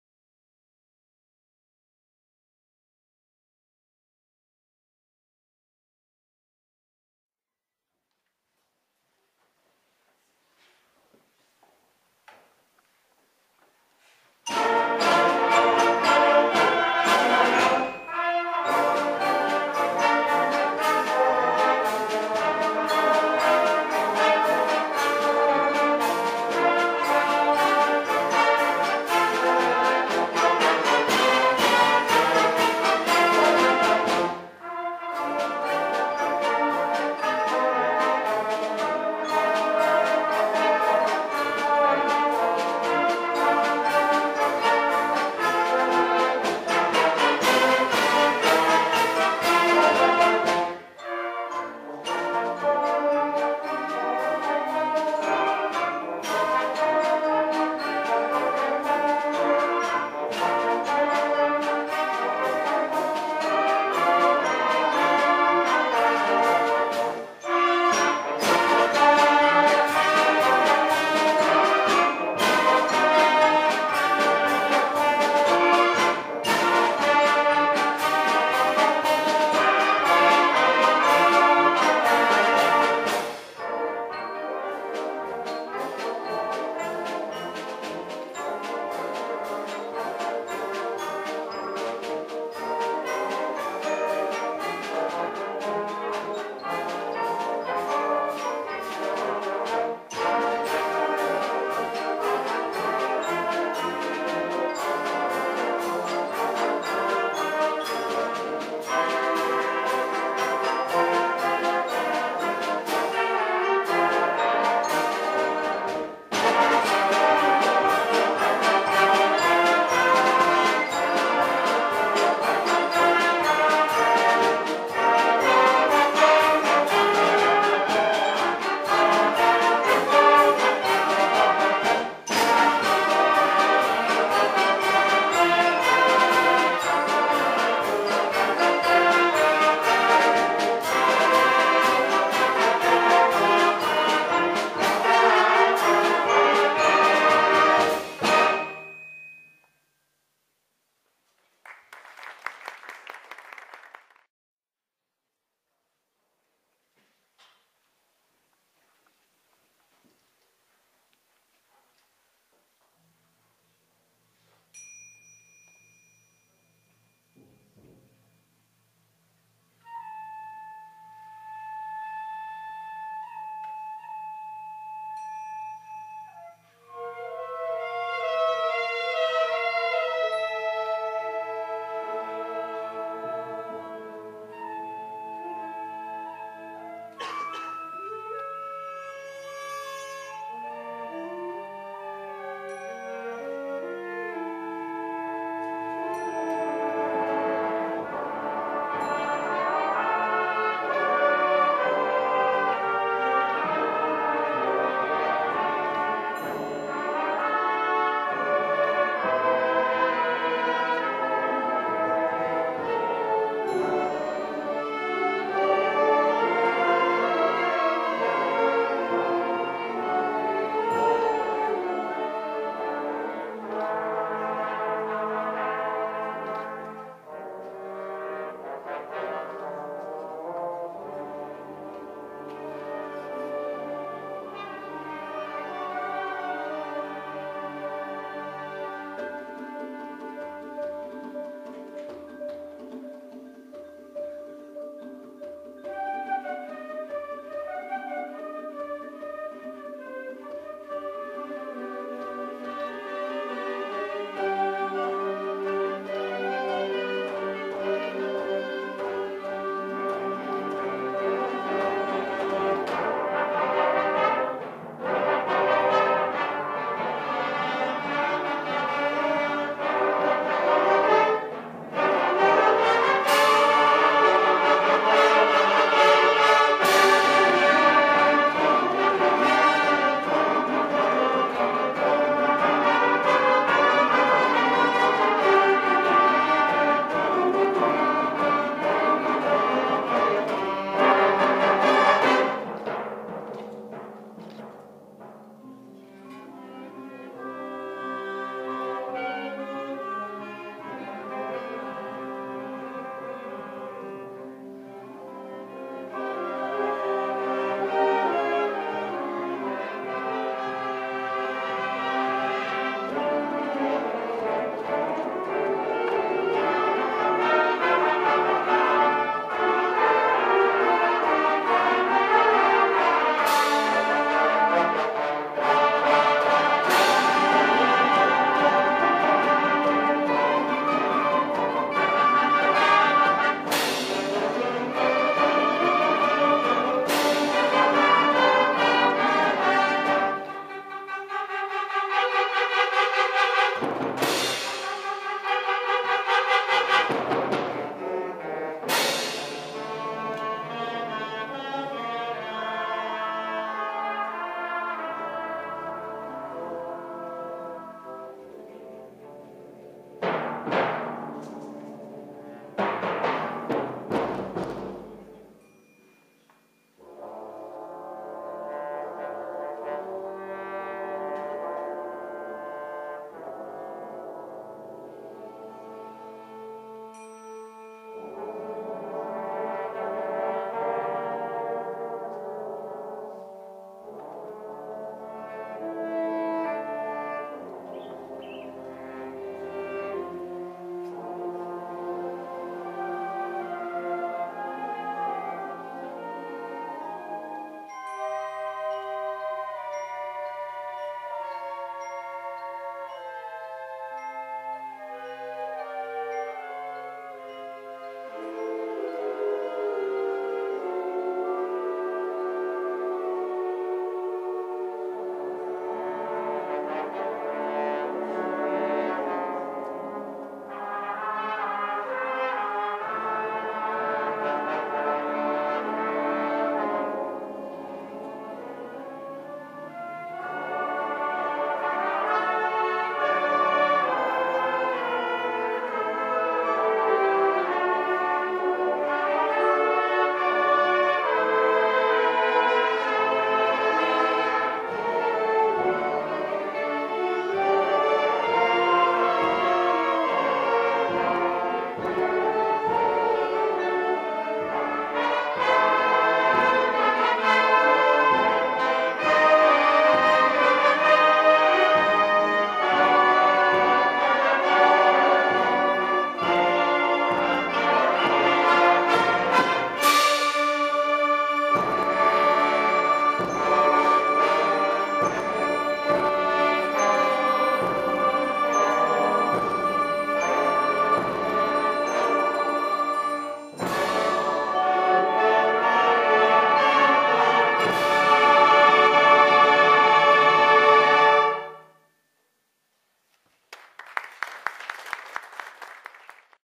2017 Warrensburg High School Concert Band
Performance at State Large Ensemble in Bonnville, MO on March 29, 2017.